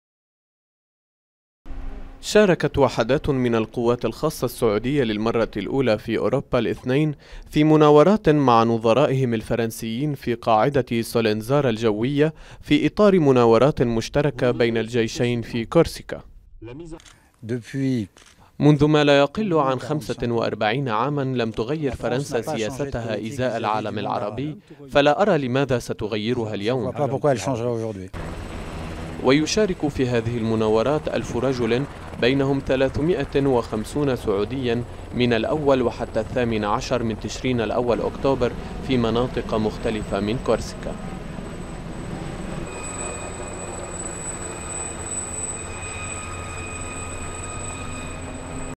مناورات للقوات الخاصة الفرنسية والسعودية في كورسيكا
شاركت وحدات من القوات الخاصة السعودية، للمرة الاولى في اوروبا، الاثنين في مناورات مع نظيرتها الفرنسية في قاعدة سولنزارا الجوية في اطار مناورات مشتركة في كورسيكا. تقرير مسجل محمد الغندور